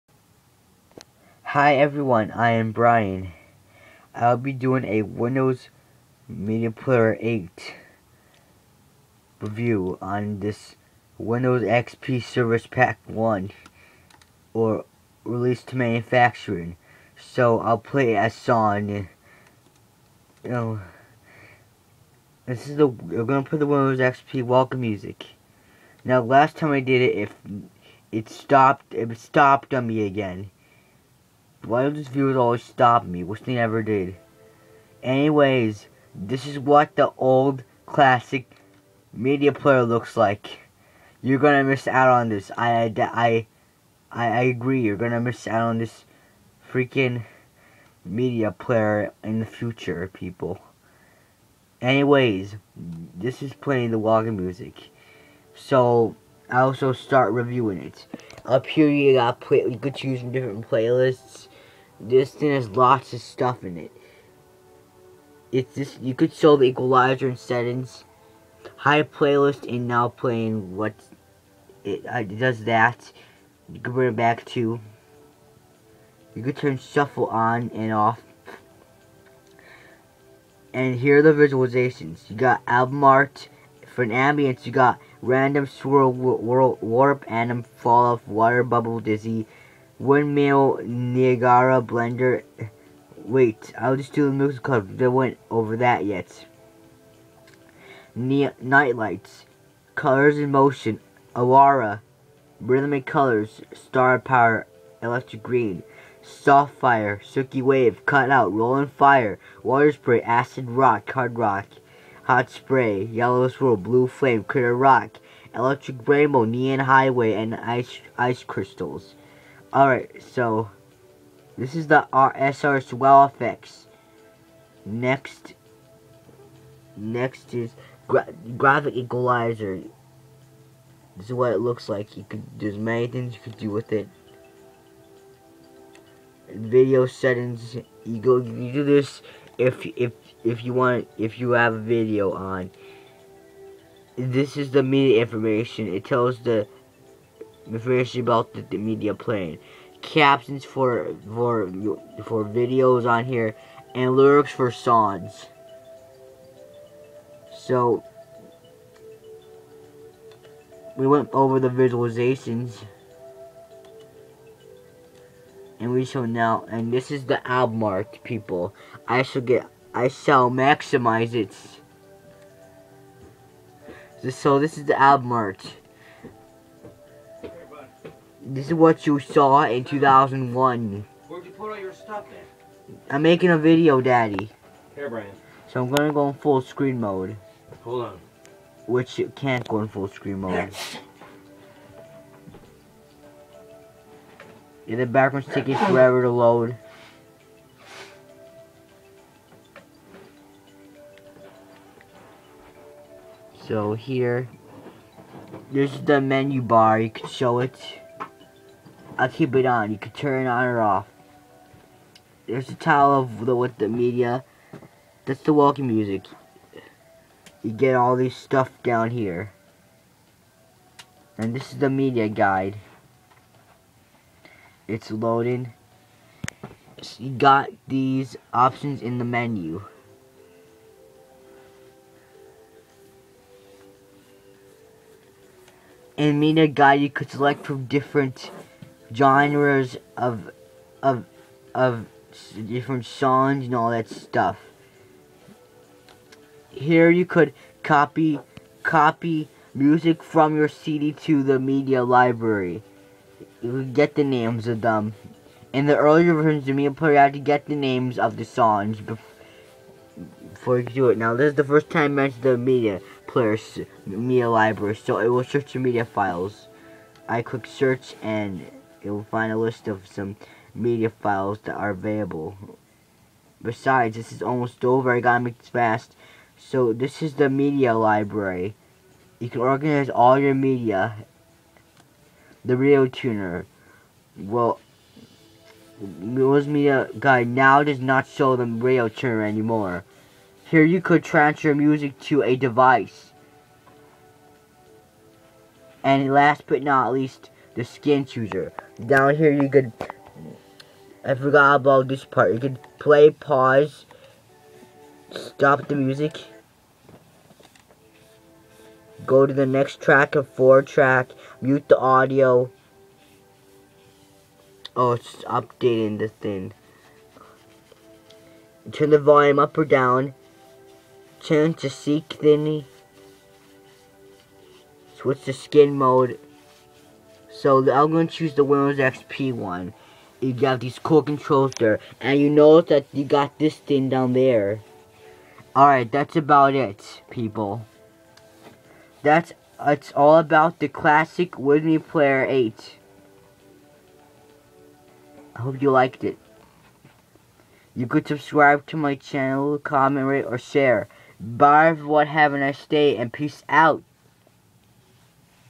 Windows Media Player for Windows XP review
This is a Windows XP review of the alternate version of Windows Media Player